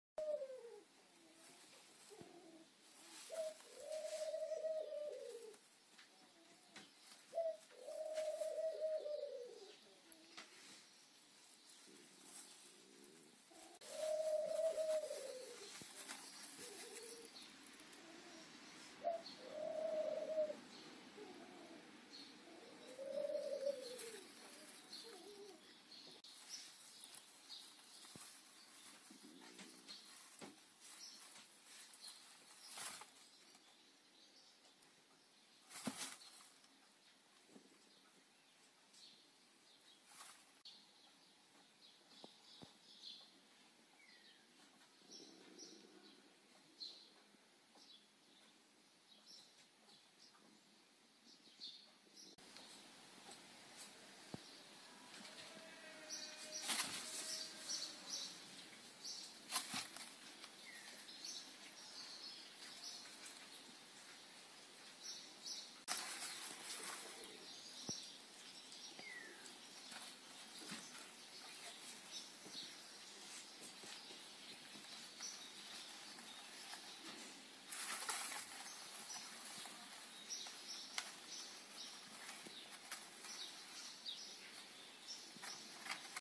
Gazapos de conejo enano: quinto día de vida
Cumplen su quinto día de vida los gazapos. Crecen y se desarrollan muy rápido.